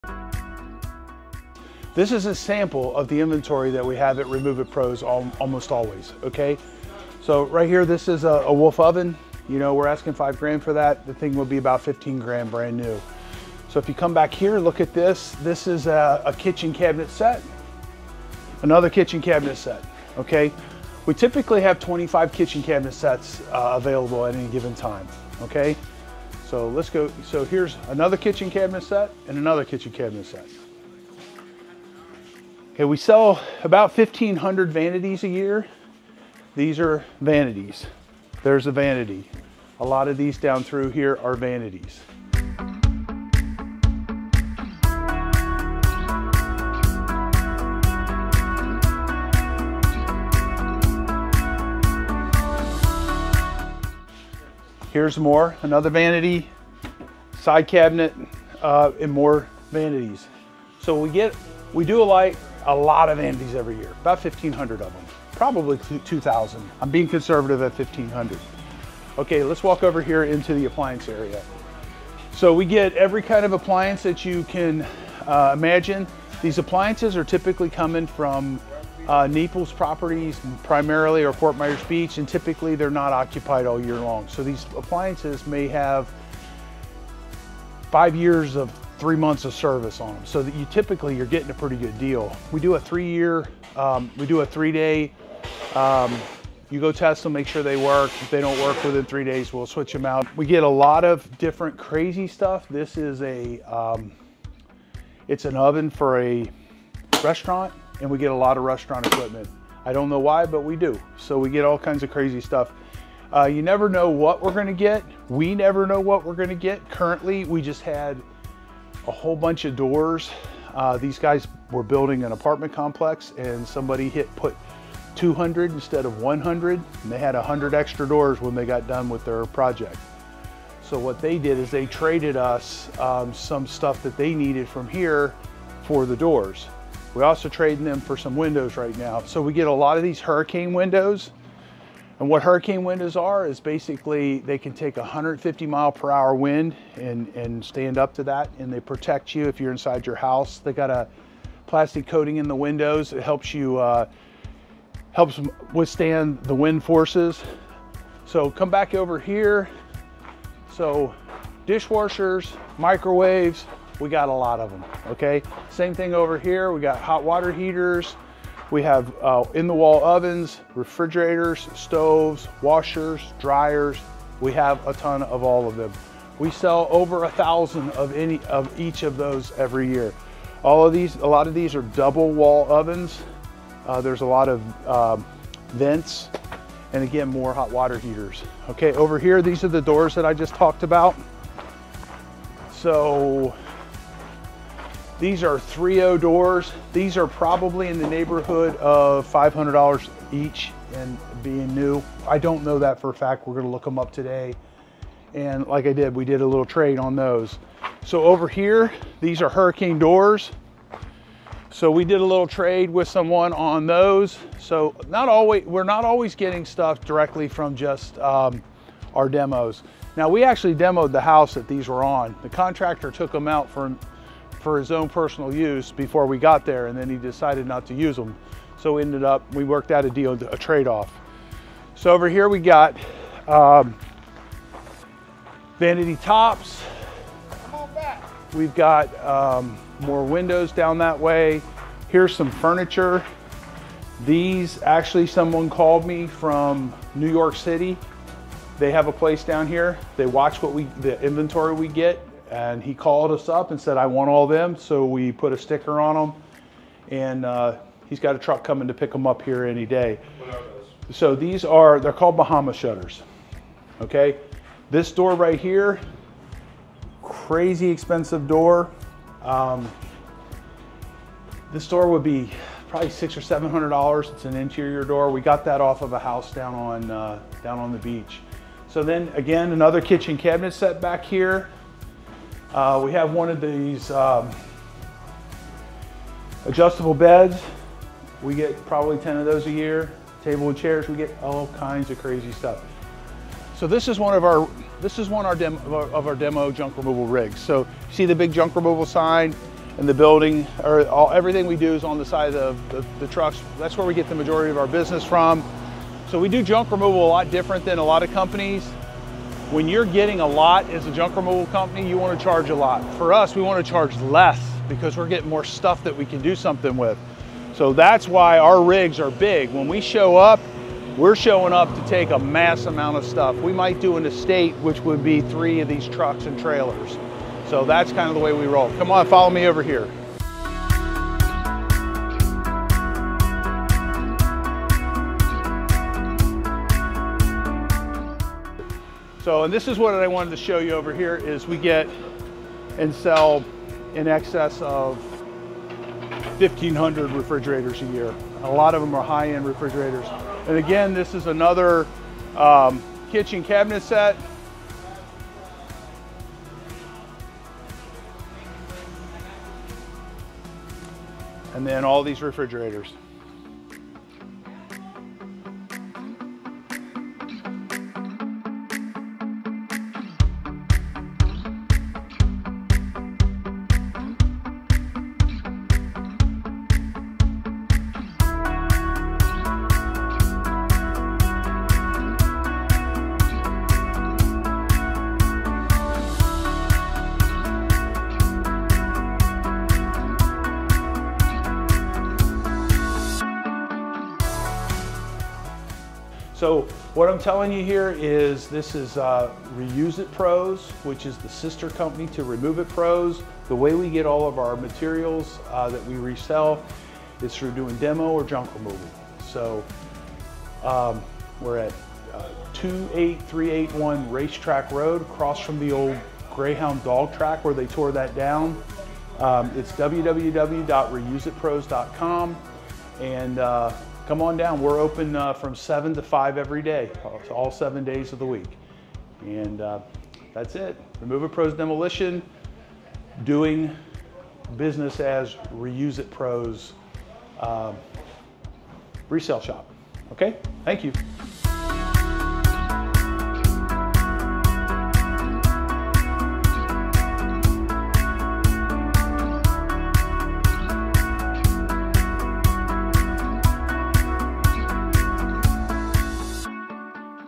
Discover Amazing Second-Hand Kitchen, Bath & Appliance Deals!
This is a sample of the inventory we have at our resale store. We are overflowing our building. Our inventory is almost updated almost daily. Lots of New items just arrived.